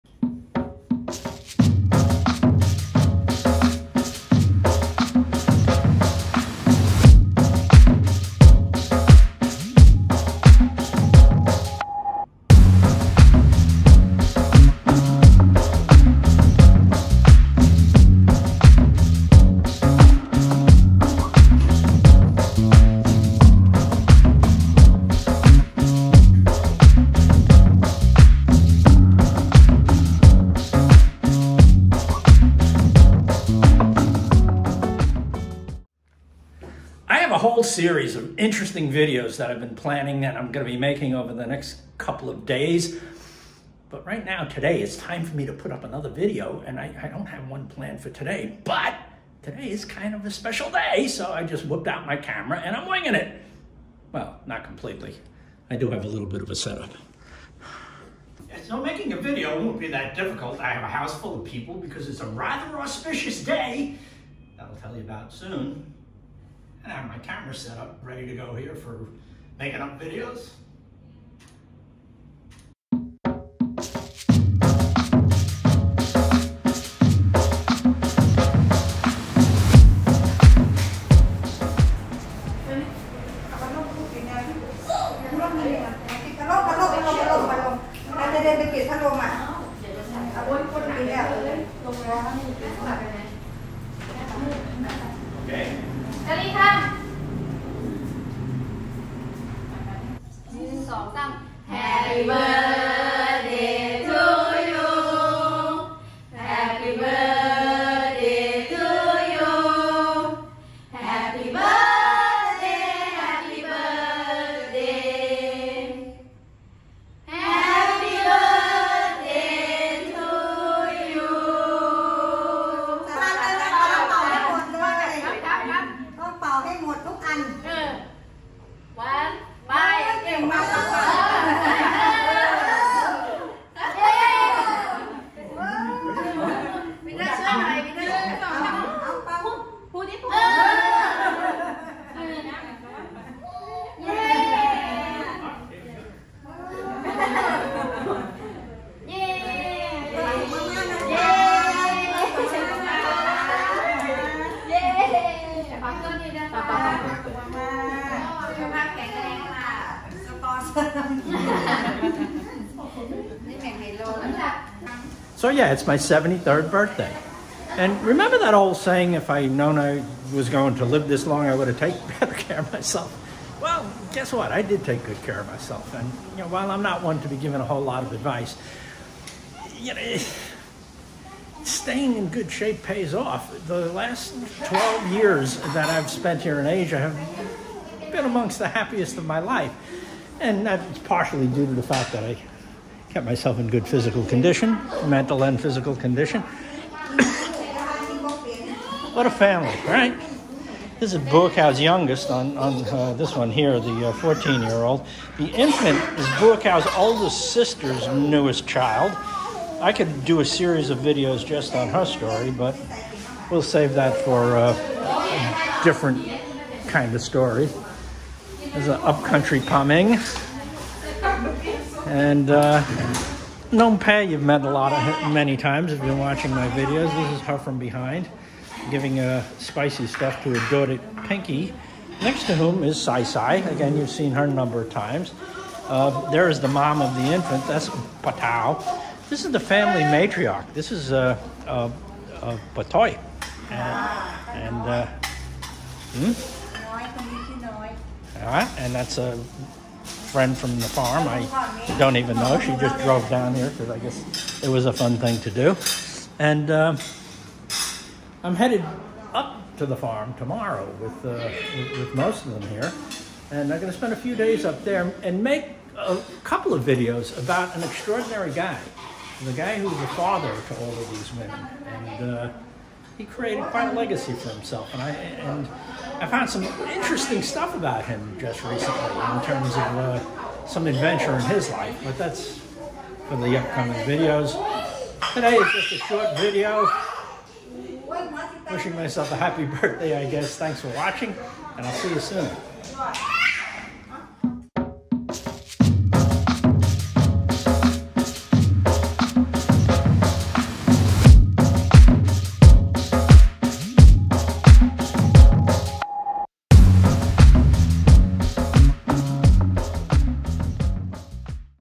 Thailand Gave Me a Birthday Present
While I have several video stories in the planning stage, tonight I'm posting a short video about my birthday.